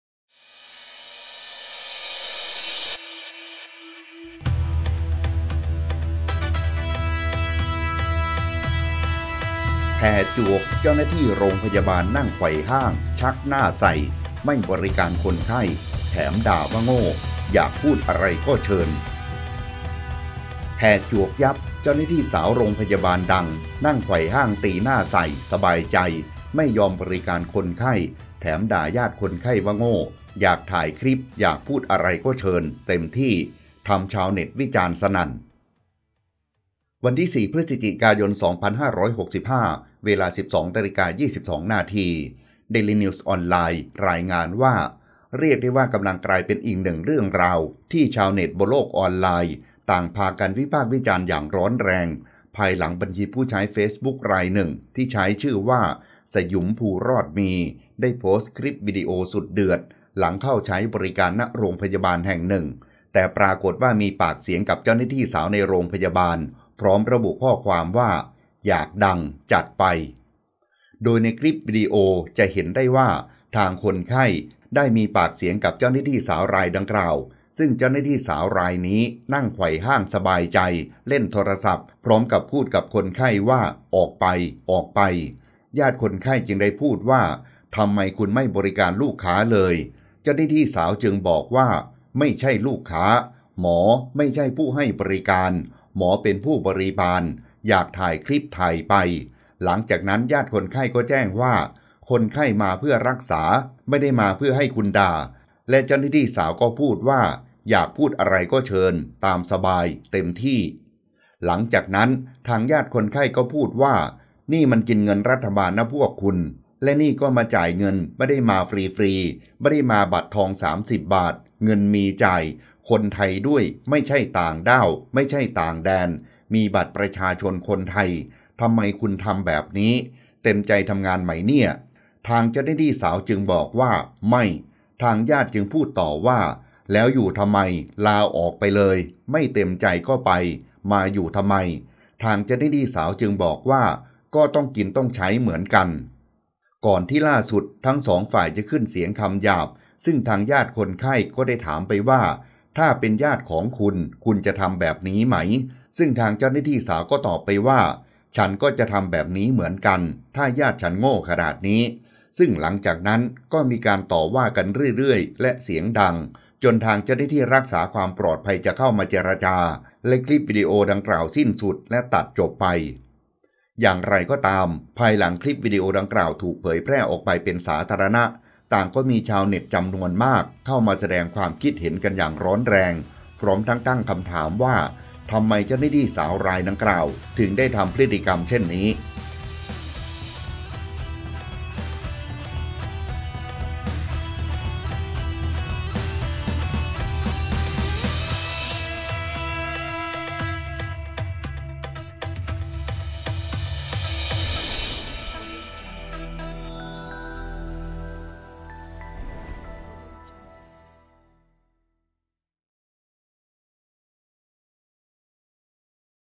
ทัวร์จอด! เจ้าหน้าที่ รพ.นั่งไขว่ห้าง ไม่บริการ ลั่น อยากพูดอะไรก็เชิญ
ขอบคุณที่มา : dailynews
ขอบคุณคลิป : สยุมภู รอดมี